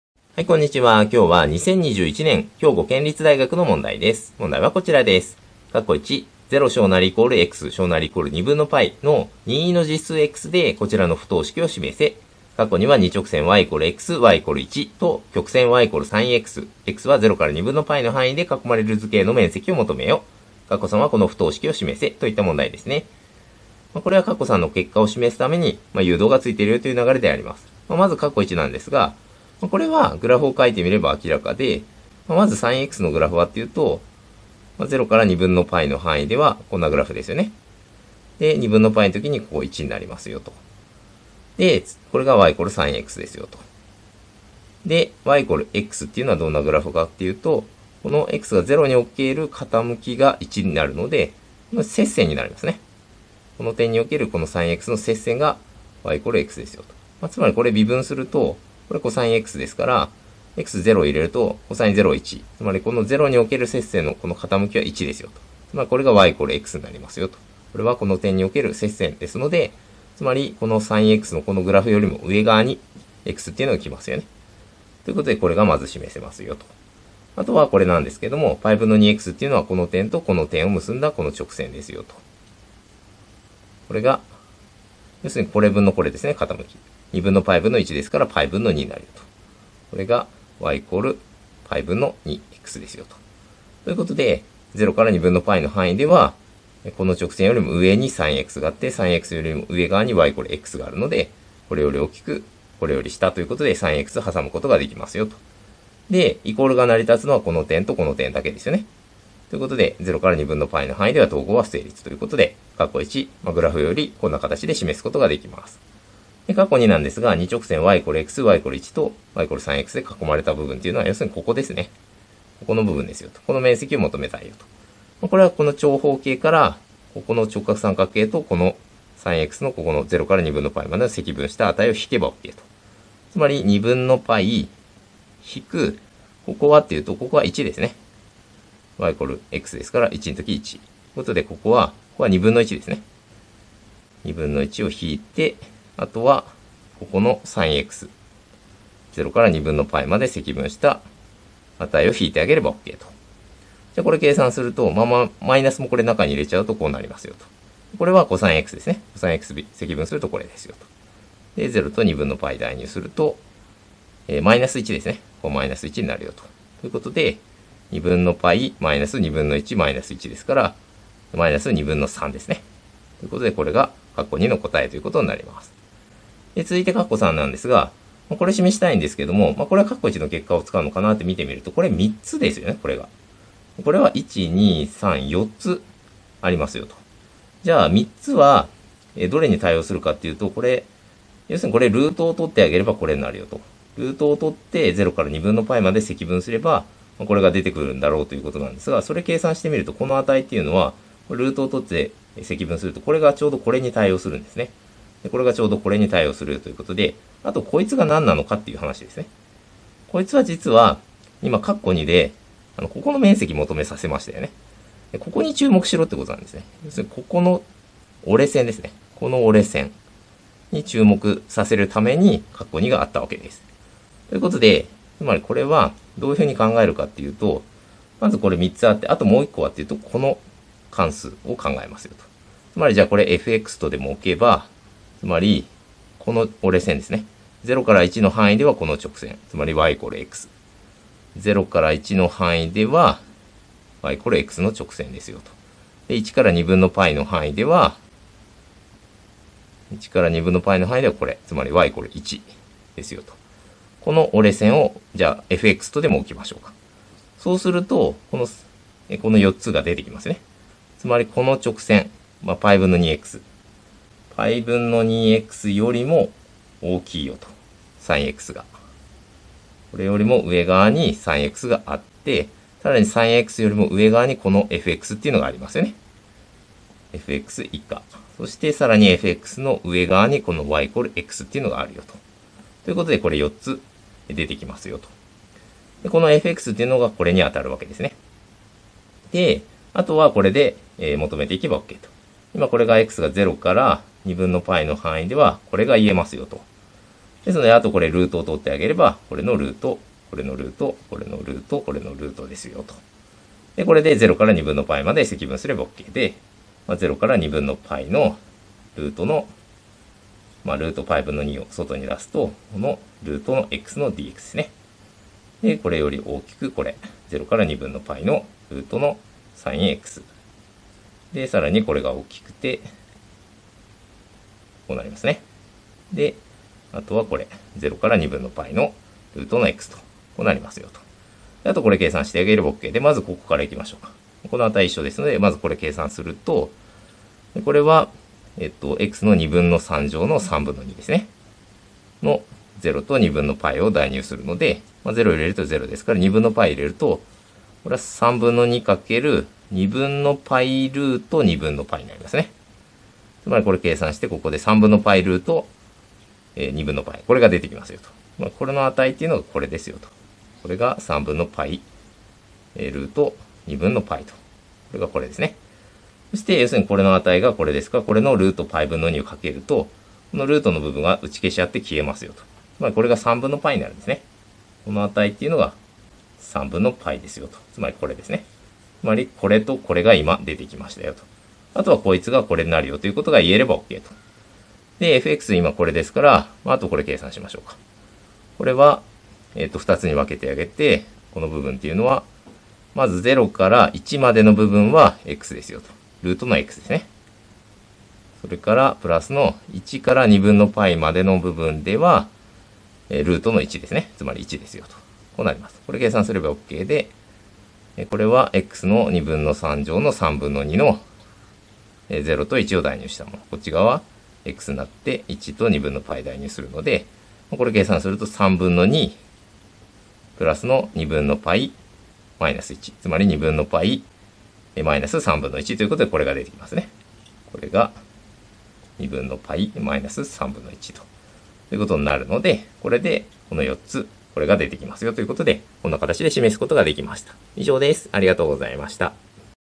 【兵庫県立大2021】不等式証明 面積による評価
y=sinxのグラフを直線ではさみ面積評価していく問題です。